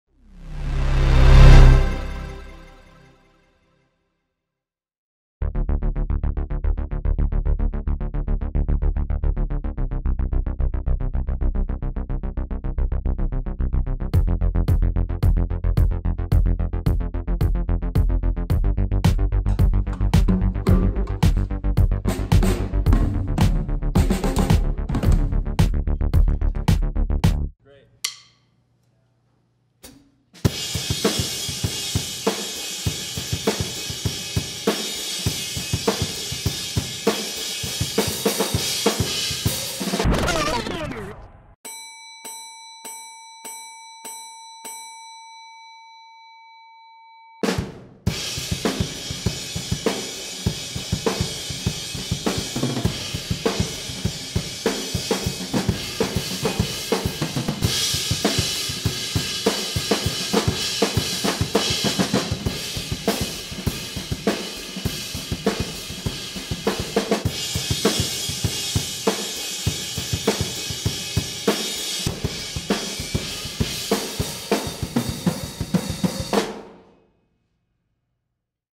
Clearsound Baffles - Product Demo (feat. Adam Christgau)
Clearsound Baffles are a transparent, lightweight acoustic treatment system for use in live music applications to control and improve sound.

Praised by both musicians and audio engineers alike.
Clearsound Baffles reduce bleed from instruments into other nearby microphones on stage and naturally dampen the delicate high-end frequencies necessary to help vocalists cut through a live mix. Unlike other acoustic treatment options, each Clearsound baffle is lightweight and can be quickly and easily mounted on a cymbal stand, then positioned wherever treatment is necessary.